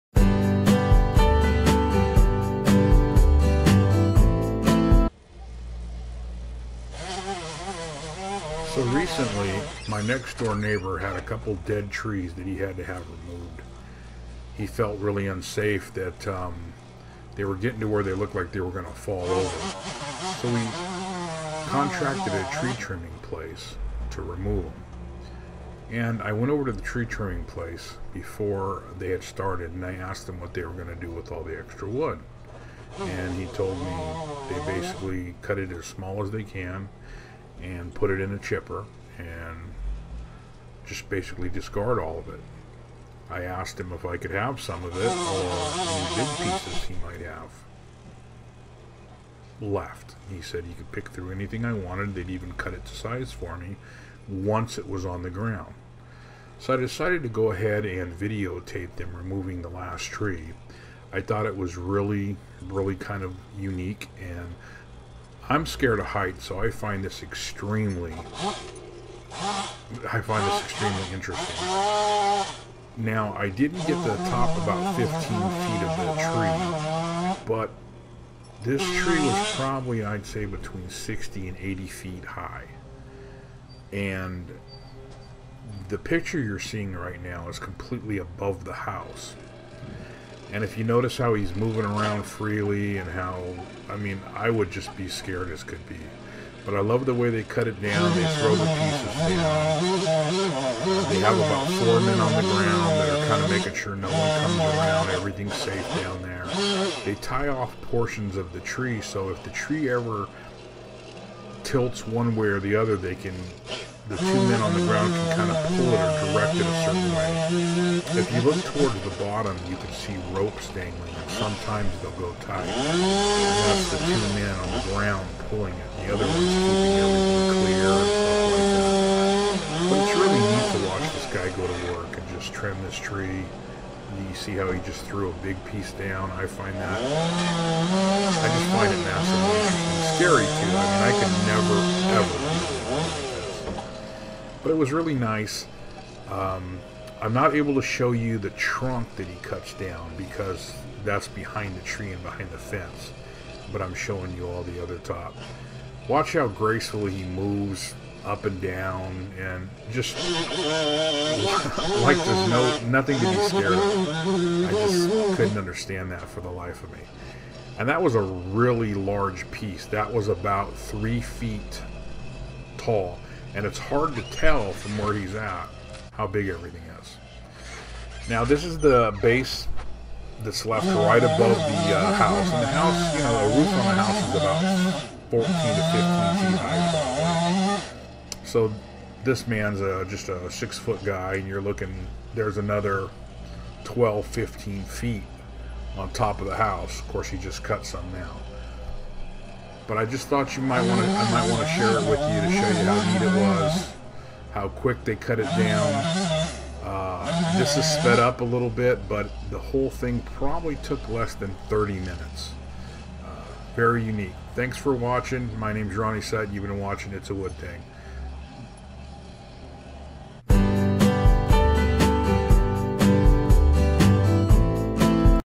Removal of a large Tree
My neighbor had a dead tree behind their house, it was rotting at the bottom, and they were scared it might fall towards the house.  I approaced the tree trimmer service, and they allowed me to have all the wood I wanted.  Most of the pieces I recieved will make great bowl blanks.